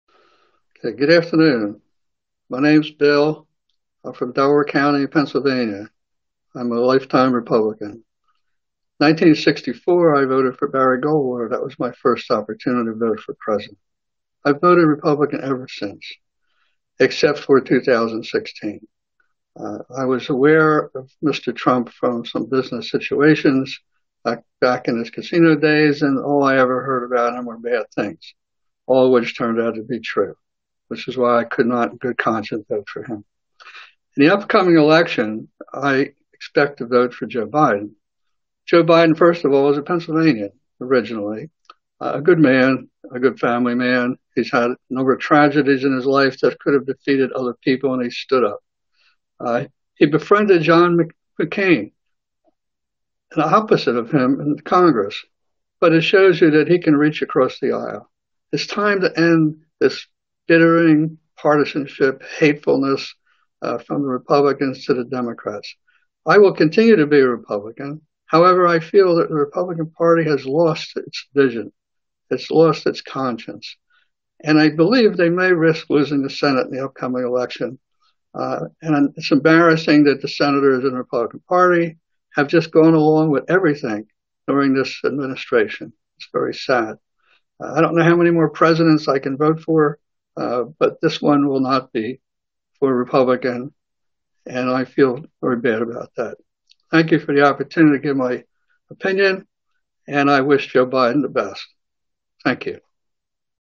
Bill from PA cast his first ballot for Goldwater in 1964 - listen to why he cannot support Trump
"I will continue to be a Republican, however I feel that the Republican party has lost its vision, it's lost its conscience, and I believe they may risk losing the Senate in the upcoming election."